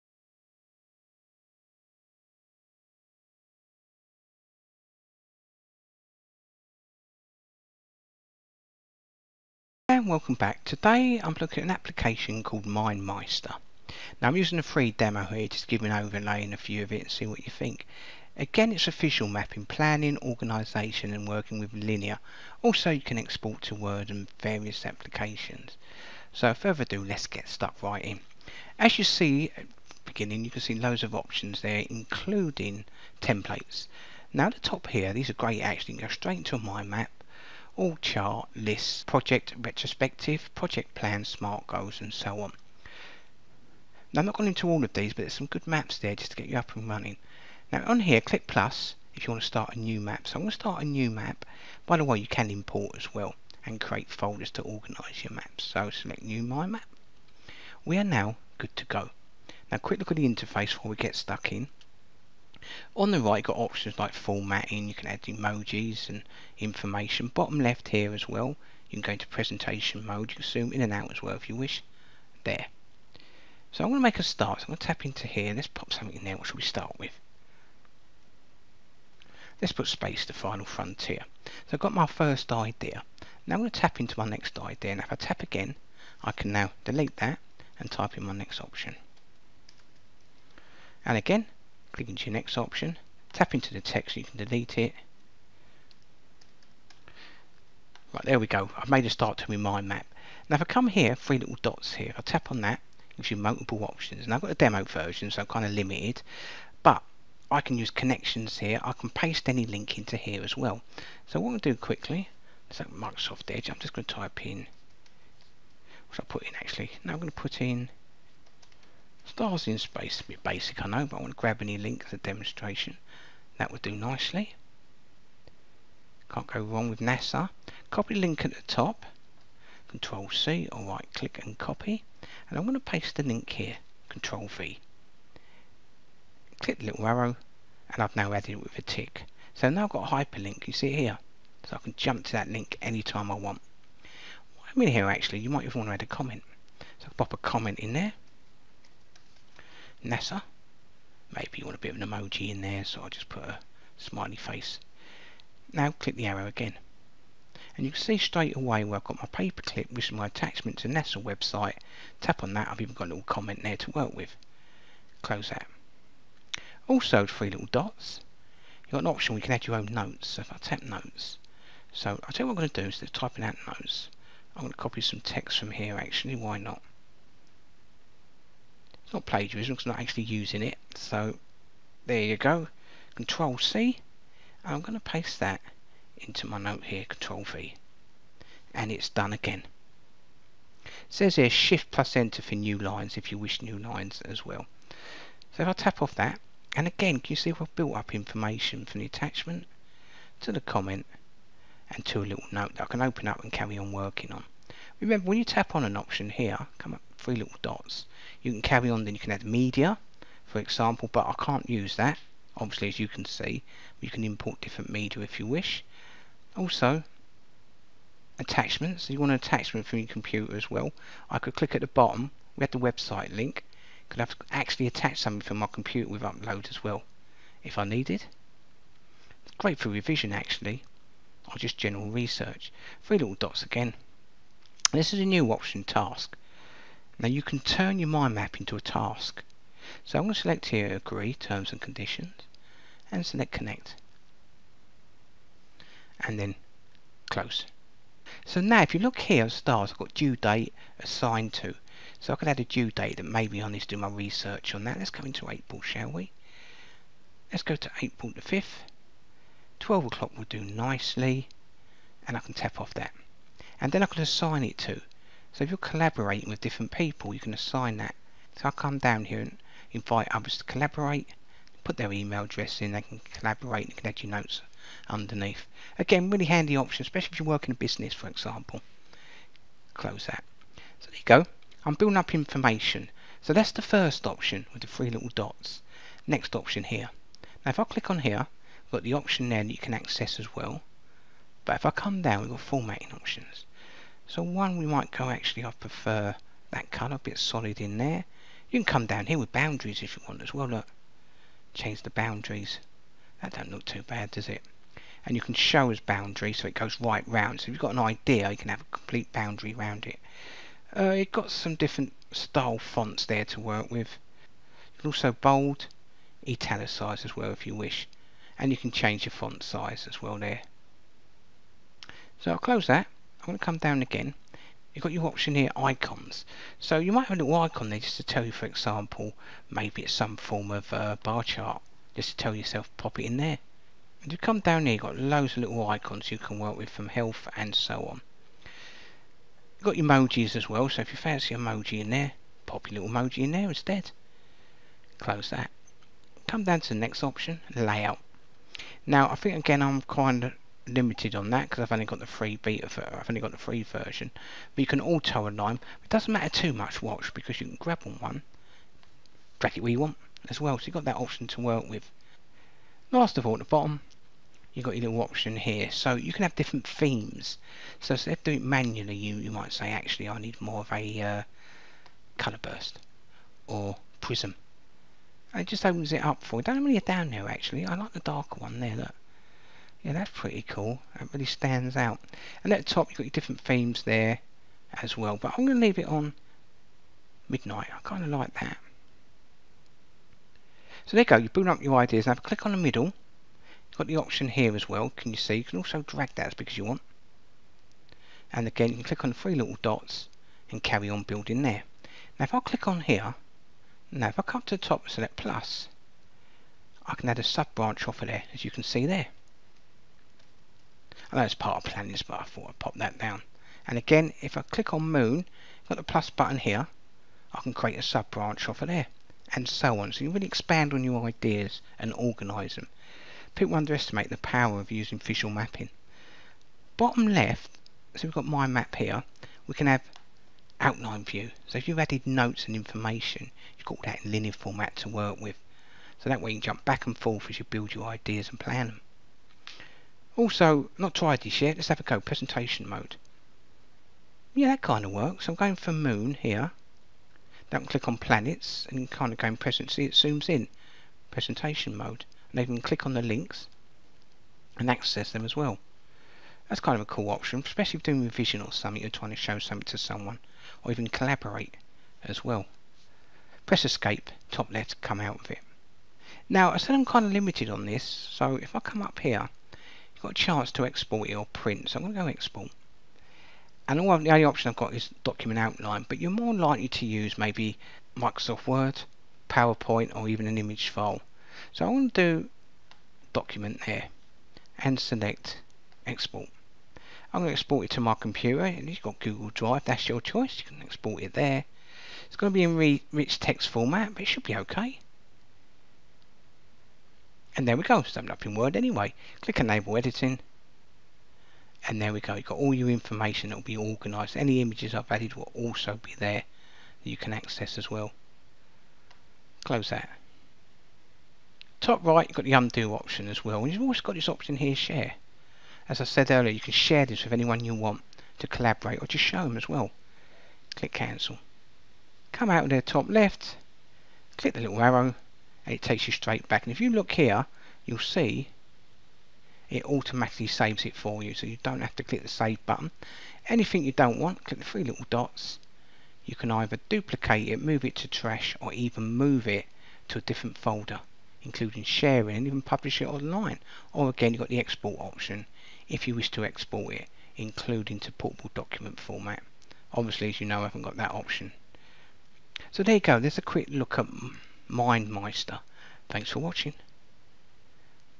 MindMeister Mapping 10 Min Tutorial
MindMeister is an online mind mapping application that allows its users to visualize, share and present their thoughts via the cloud. MindMeister was launched in 2007 by MeisterLabs GmbH, a software company founded by Michael Hollauf and Till Vollmer.